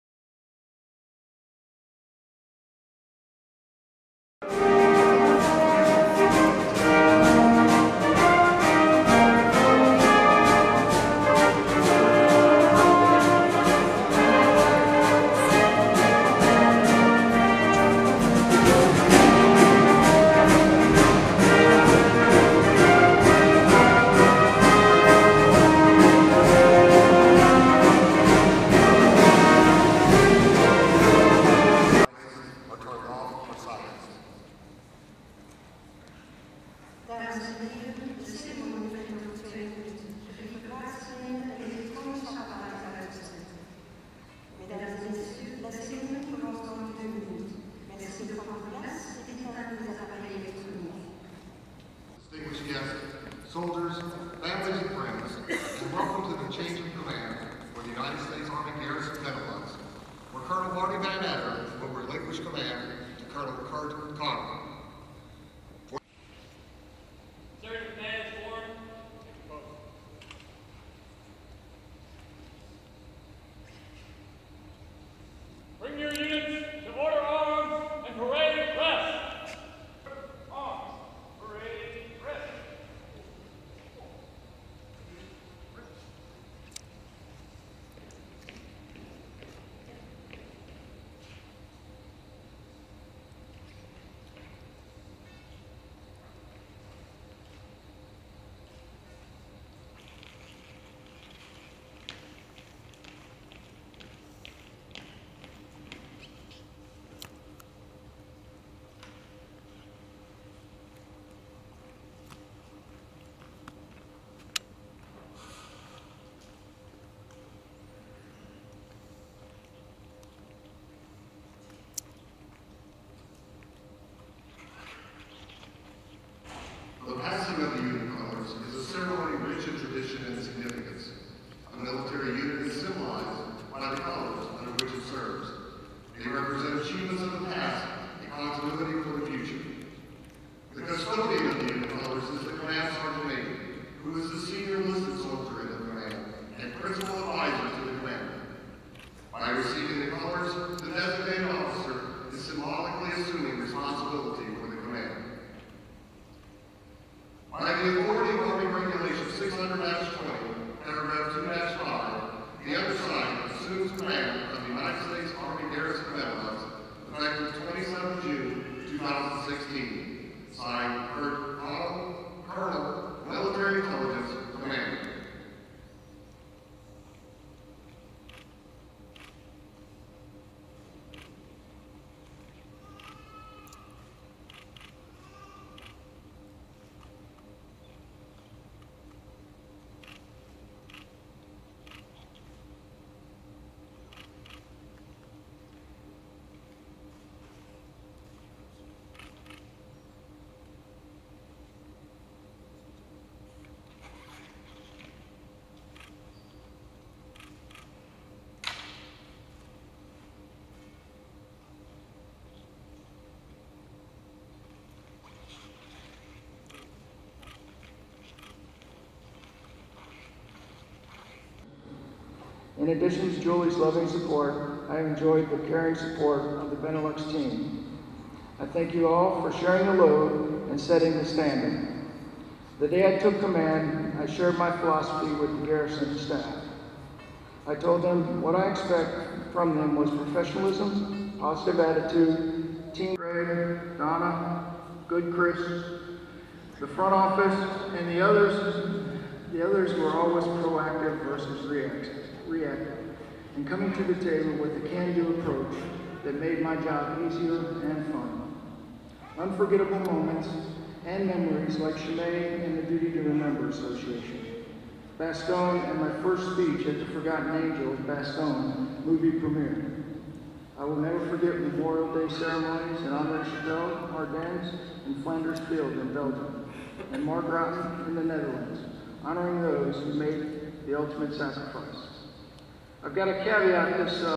160627-A-RX599-901
Change of Command of U.S Army Garrison Benelux, between Col. Narty G. Vannatter (outgoing Commander) and Col. Kurt P. Connell (incoming Commander) in the community activity center building 4 , on Chievres Air Base, Belgium, June 27, 2016. (U.S. Army video by Visual Information Specialist Pascal Demeuldre/released) [779RHH1_DES9VRG45_fc.mov]